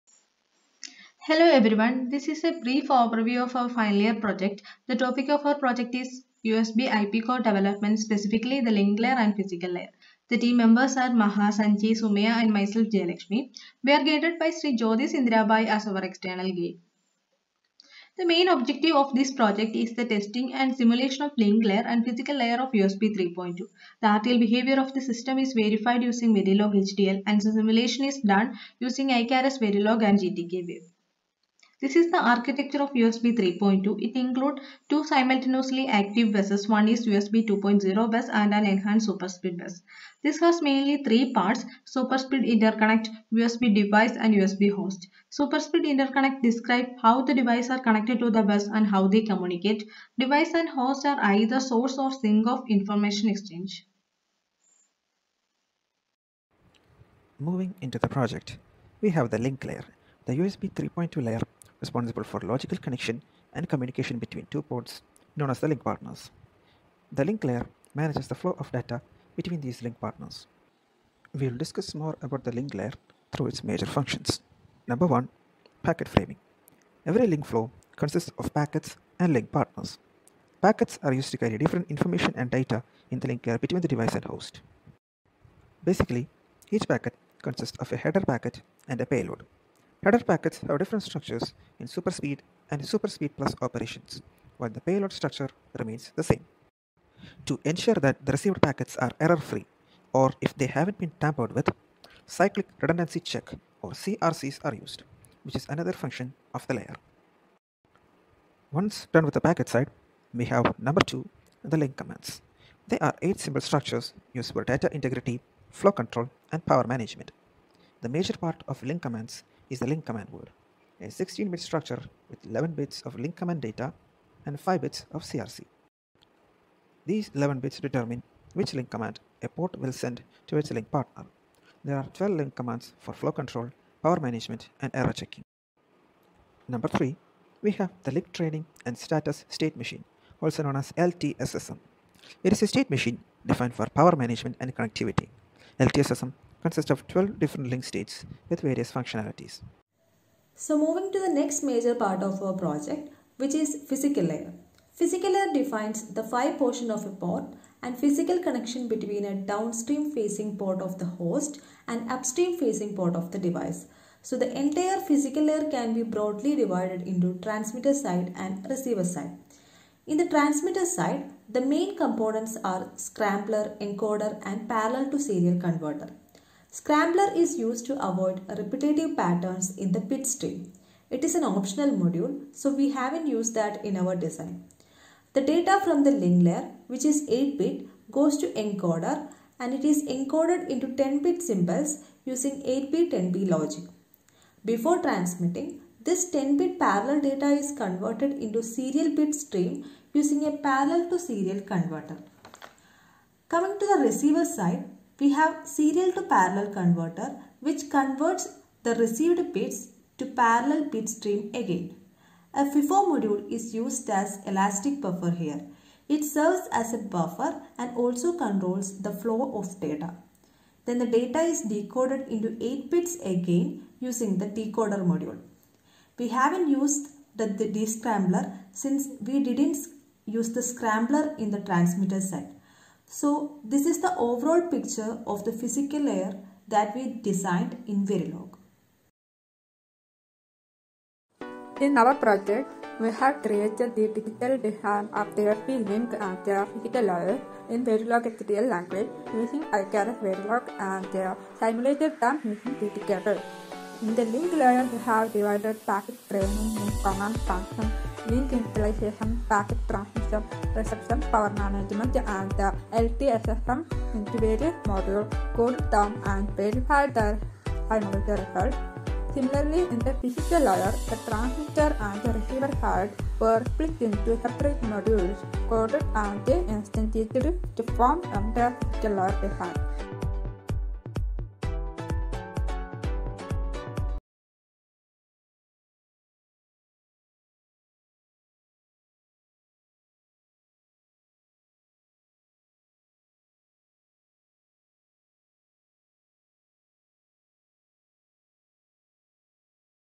USB IP Core Development(Phy & Link Layers)
Final year project by:
Jayalakshmi V S (TVE17EC021)
Maha Ahamad P T (TVE17EC024)
Sanjay S Nair (TVE17EC041)
Sumayya A K (TVE17EC055)

Under the guidance of Dr. Jyothis Indirabhai, Director, Netrasemi Pvt. Ltd., Trivandrum